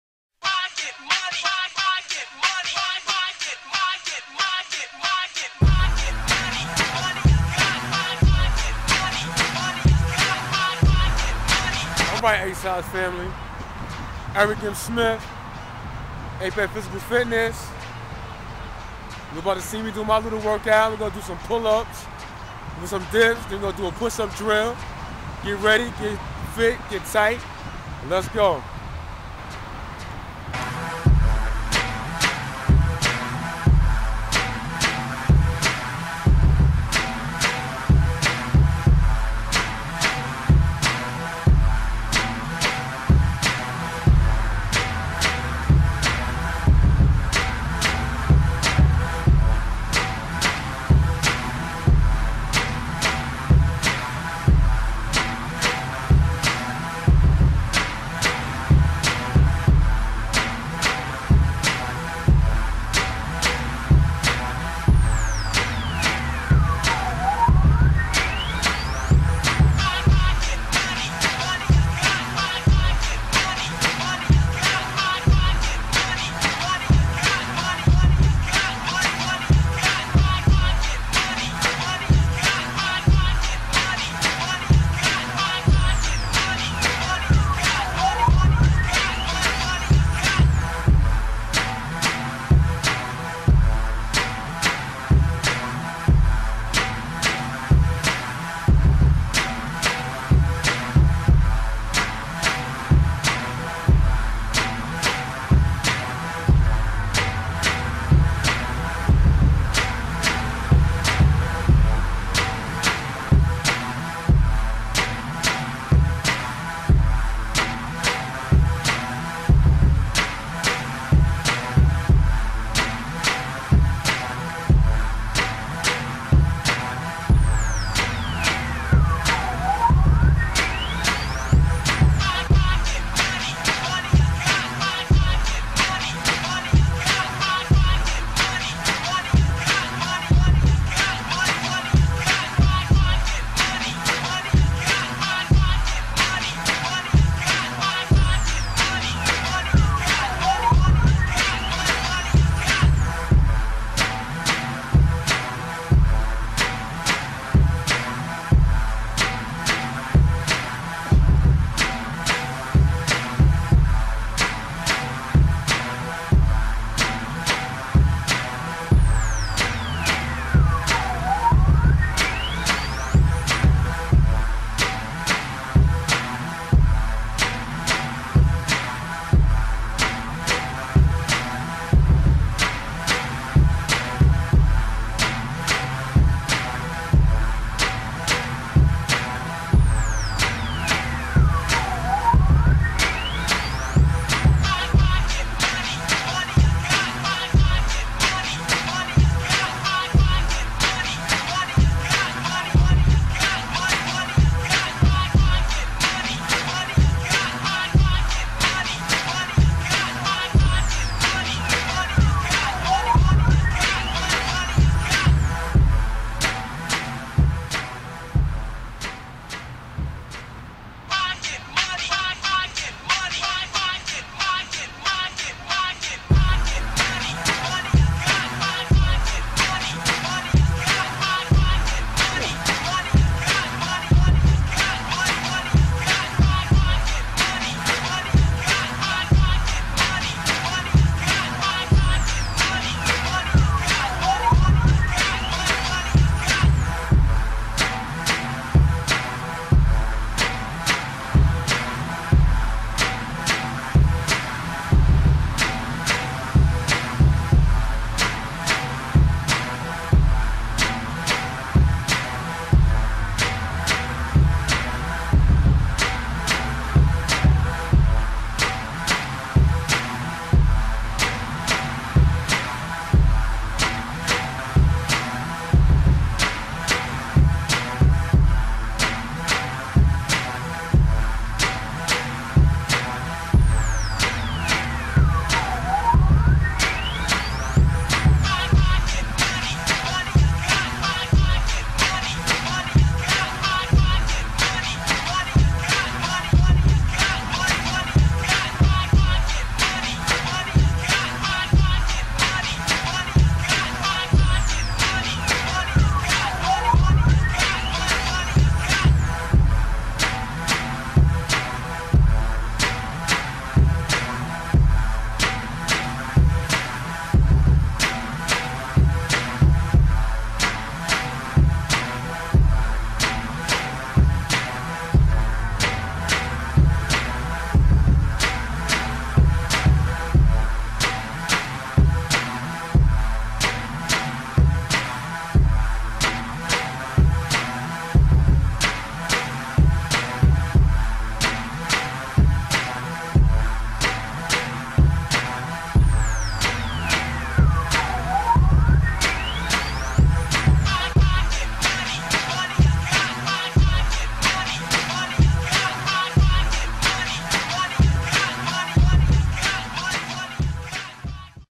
My Work Out, Strength Training(Apex Physical Fitness)
Erick M. Smith shows you another way of getting fit